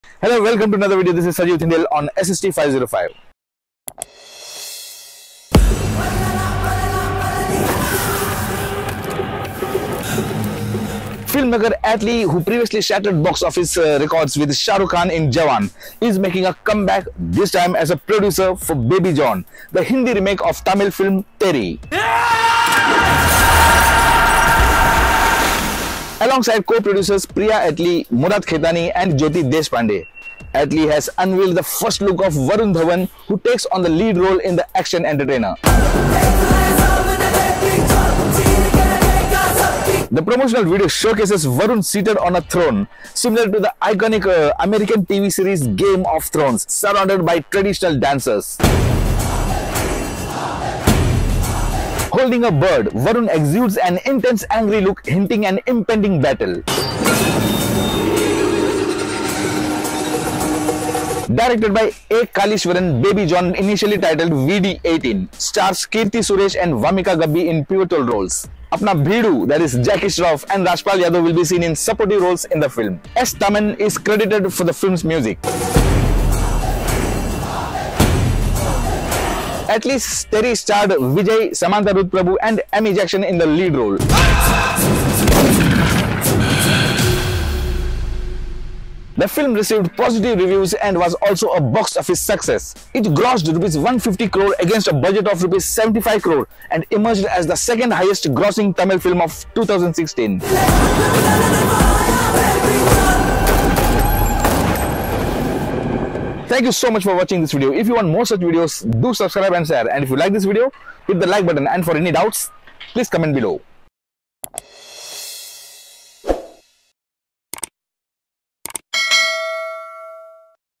Baby John Teaser Talk | Biggest Action Entertainer | Varun Dhawan, Keerthy Suresh & Wamiqa Gabbi
Baby John Teaser Talk | Biggest Action Entertainer | Varun Dhawan, Keerthy Suresh & Wamiqa Gabbi@sajeevthindiyel